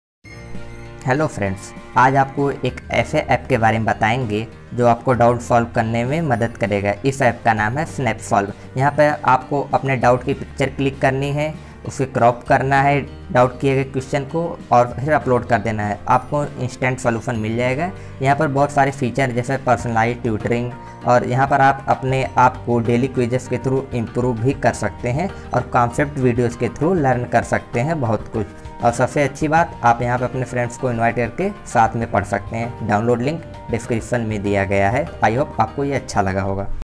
Best Doubt Solving App| Solve PCMB doubts in an Instant | Snapsolve
SnapSolve is a free doubt-solving app that provides instant solutions to Math, Physics, Chemistry, Biology & Science doubts and NCERT book questions.